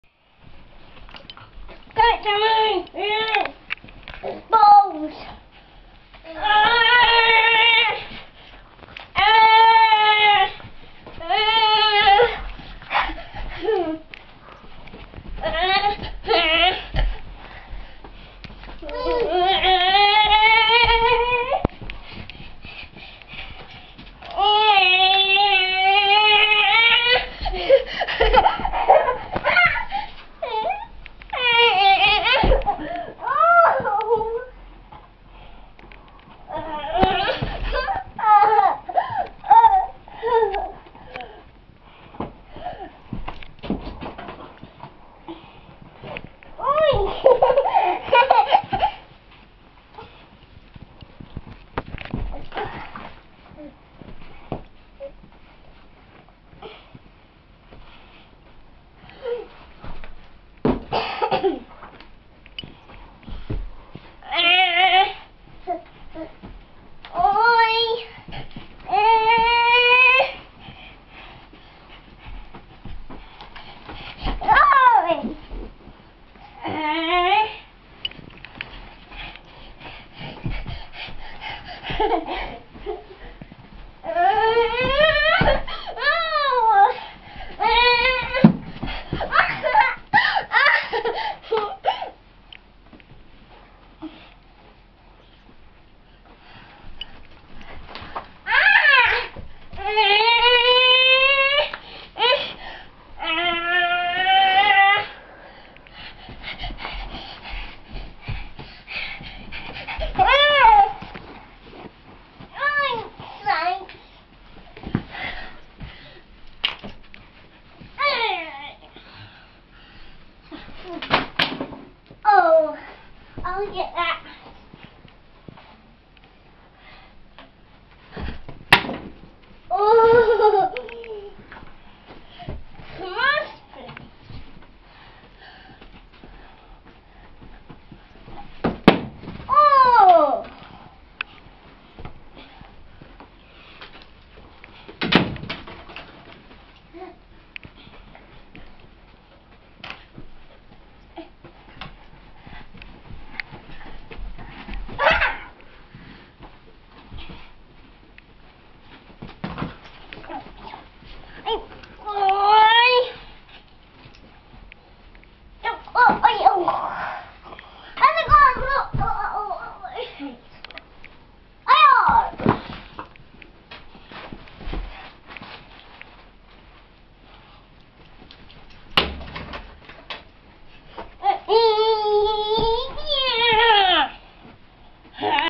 Border collie and child play with a frisbee
Holly my border collie, playing frisbee with my nephew Liam.